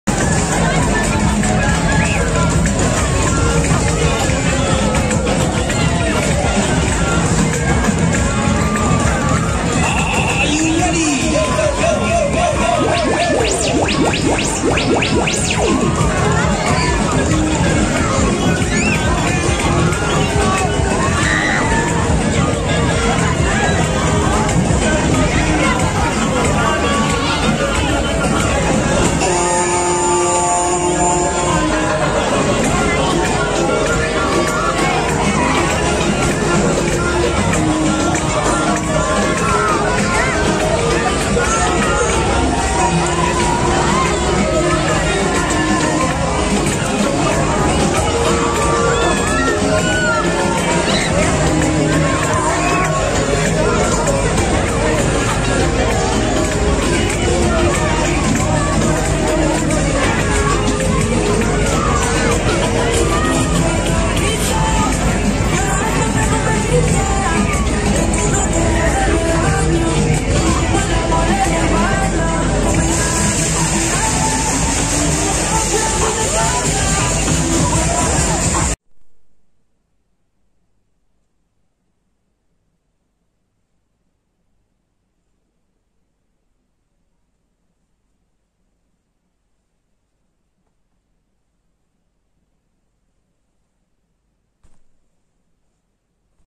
The King - Feria de invierno de Sabadell 2021-2022
Esta Feria Se Encuentra En el parc central del Vallès, en Sabadell. Y el último día es el 9 de enero de 2022